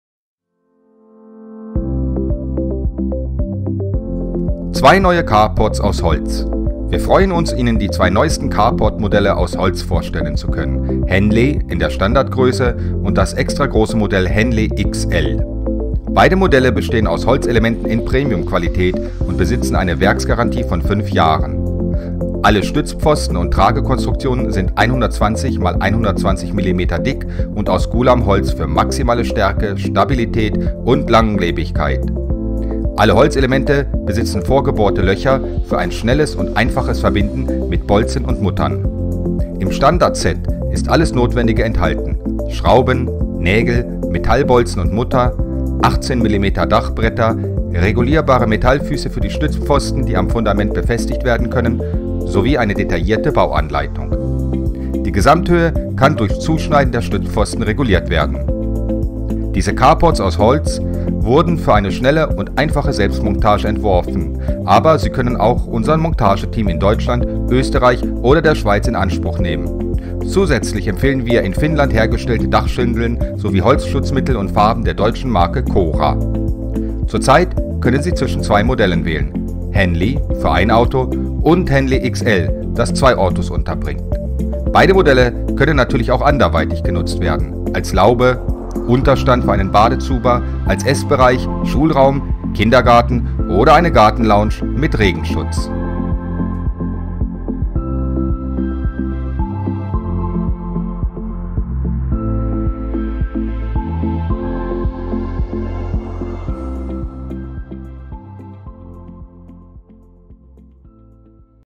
Zwei neue Carports aus Holz
Wir freuen uns, Ihnen die zwei neuesten Carport-Modelle aus Holz vorstellen zu können: „Henley“ (Standardgröße) und das extragroße Modell „Henley XL“.

Beide Modelle bestehen aus Holzelementen in Premium-Qualität und besitzen eine Werksgarantie von 5 Jahren. Alle Stützpfosten und Tragekonstruktionen sind 120×120 mm dick und aus Gulam-Holz für maximale Stärke, Stabilität und Langlebigkeit. Alle Holzelemente besitzen vorgebohrte Löcher für ein schnelles und einfaches Verbinden mit Bolzen und Muttern. Im Standardset ist alles Notwendige enthalten: Schrauben, Nägel, Metallbolzen und -mutter, 18 mm Dachbretter, regulierbare Metallfüße für die Stützpfosten, die am Fundament befestigt werden können, sowie eine detaillierte Bauanleitung.